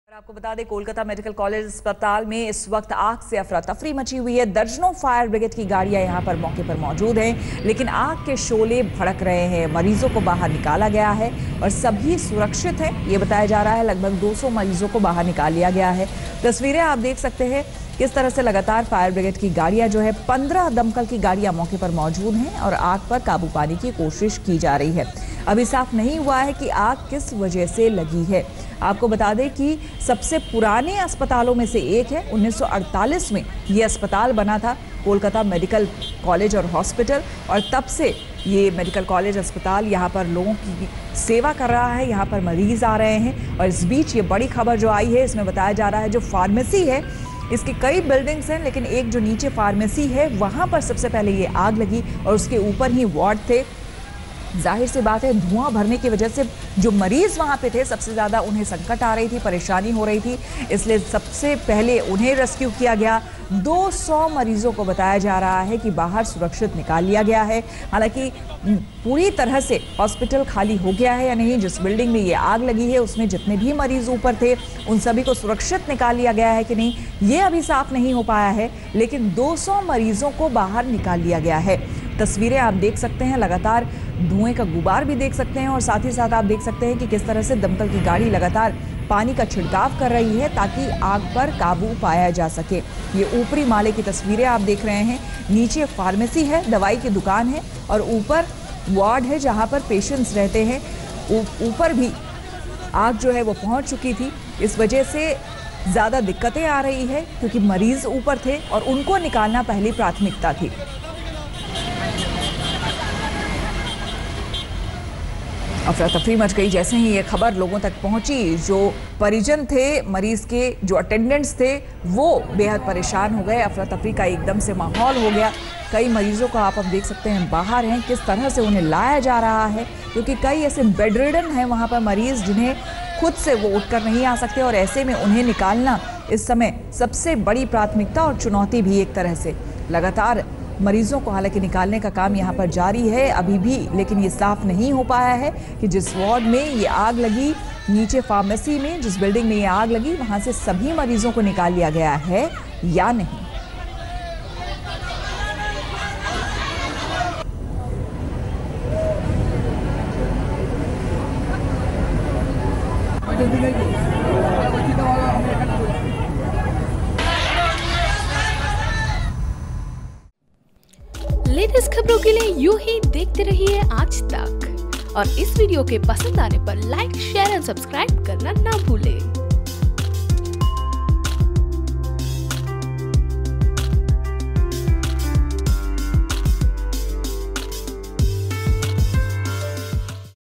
Kolkata की Medical College में लगी भीषण आग, 200 से ज्यादा मरीजों को सुरक्षित निकाला गया
A massive fire broke out in pharmacy store of a medical college in Kolkata. More than 200 patients were rescued by the authorities. Till now no casualty is reported.